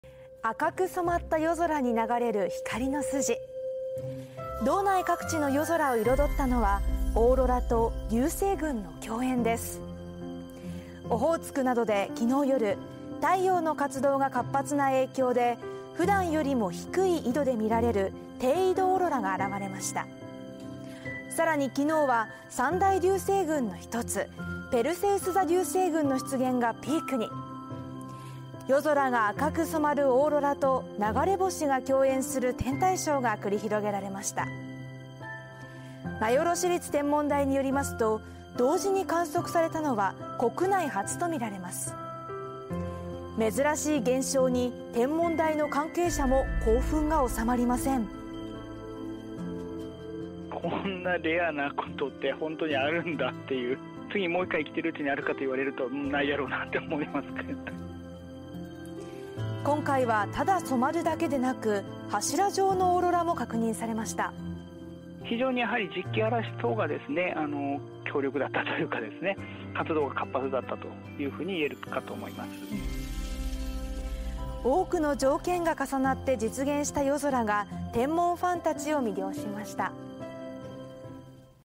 "低緯度オーロラ"＆"ペルセウス座流星群"…共演は『国内初』か 「こんなレアなことが本当に」関係者からも驚きの声 強い磁気嵐で柱状も確認 北海道
赤く染まった夜空に流れる光の筋。

　北海道内各地の夜空を彩ったのはオーロラと流星群の共演です。

　オホーツクなどで8月12日夜、太陽の活動が活発な影響で普段よりも低い緯度で見られる「低緯度オーロラ」が現れました。

　さらに、8月12日は三大流星群の一つ「ペルセウス座流星群」の出現がピークに。夜空が赤く染まるオーロラと流れ星が共演する天体ショーが繰り広げられました。

　なよろ市立天文台によりますと、同時に観測されたのは国内初とみられます。

　珍しい現象に天文台の関係者も興奮が収まりません。

　「こんなレアなことって本当にあるんだ。もう1回生きてるうちにあるかと言われるとないだろう」（なよろ市立天文台 村上恭彦 台長）

　今回はただ染まるだけでなく、柱状のオーロラも確認されました。

　「（柱状のオーロラは）非常に磁気嵐が強力だったというか、活動が活発だったと言えると思う」（なよろ市立天文台 村上恭彦 台長）

　多くの条件が重なって実現した夜空が多くの天文ファンを魅了しました。